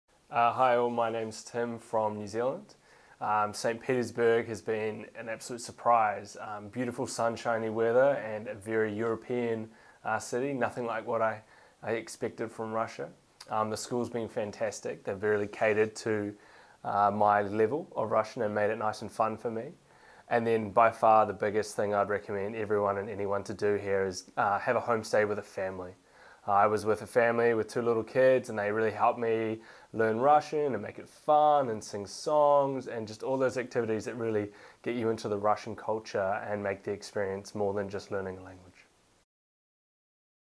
Exlinguo St Petersburg school review: Timothy from New Zealand (April 2019)
Timothy is from New Zealand and was looking for an intensive Russian course in St Petersburg to build confidence in the language. His partner is originally from Russia and he wanted to be able to speak conversational Russian with her and her family. His 2-week course helped him get a more solid foundation in Russian language and staying with a local host family gave him an insight in Russian culture and daily life in Russia.